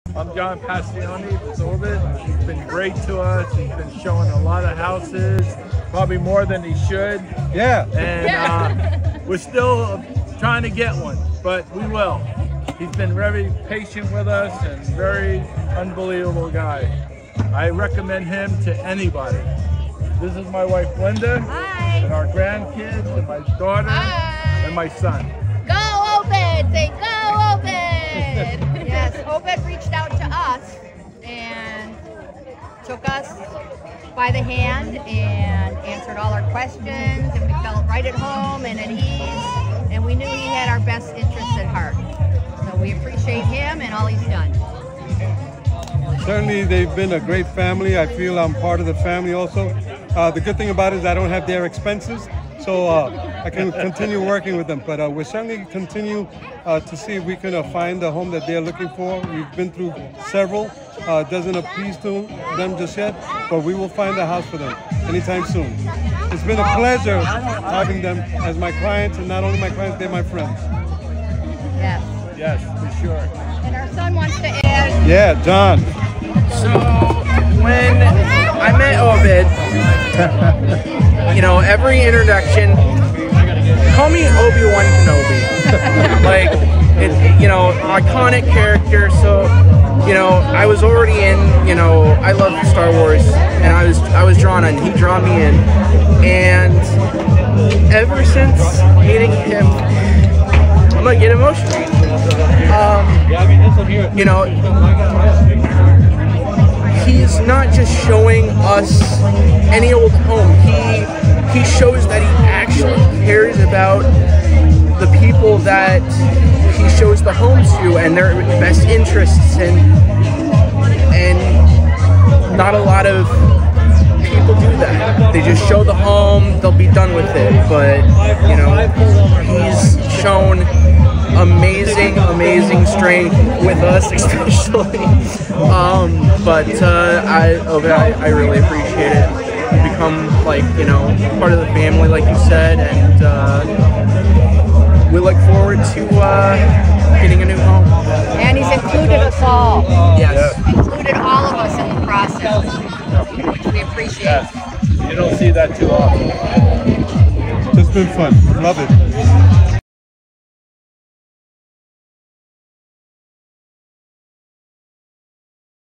Client Testimonial
Gratitude in Every Word 🌟 Thank you to our amazing client for sharing the love and appreciation for Obed, our stellar agent at MVP Home Group. Your testimonial fuels our passion for excellence! 🏡💙